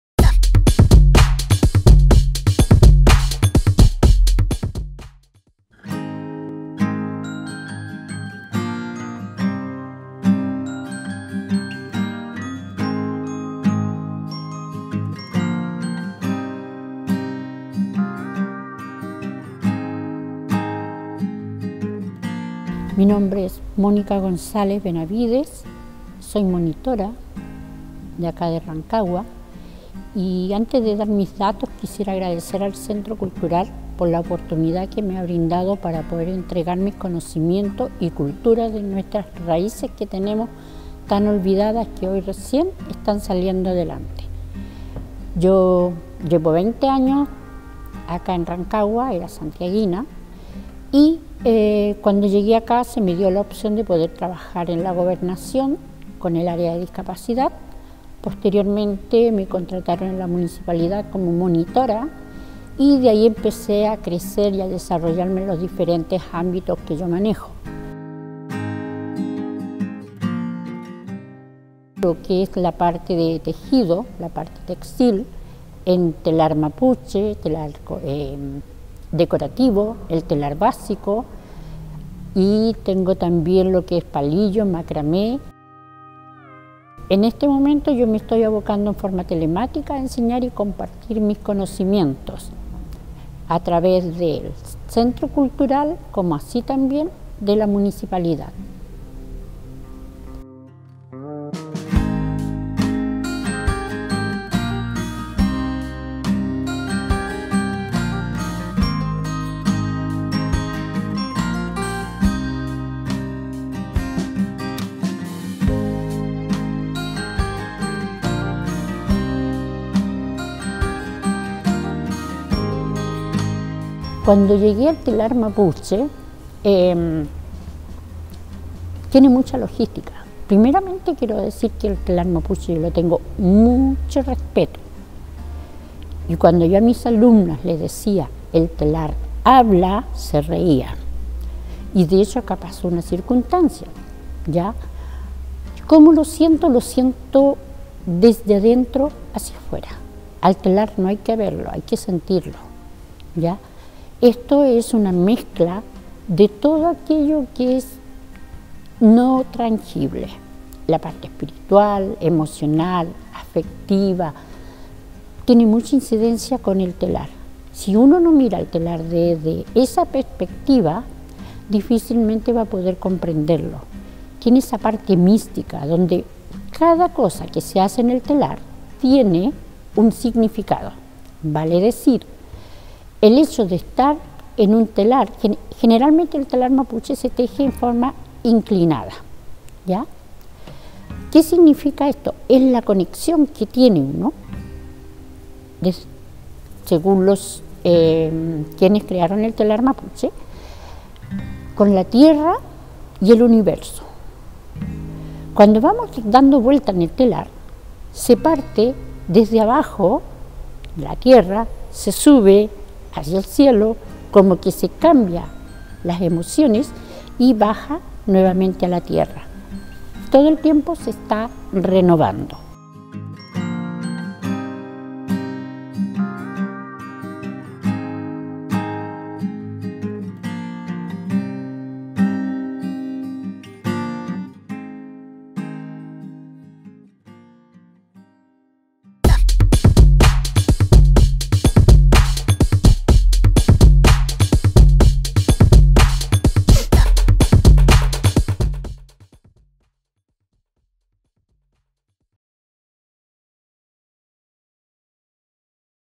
MÓNICA GONZÁLEZ / ARTESANA TEJIDOS ANCESTRALES
Un rudimentario marco de madera, ovillos de lana y unas manos cálidas y pacientes evocan la ancestral técnica conocida como Telar Mapuche.

Un sinfín de lanas de vivos colores, mucha creatividad y dedicación son parte de las herramientas necesarias que la artesana Mónica González utiliza para dar vida a hermosas piezas textiles como murales, alfombras y prendas de vestir.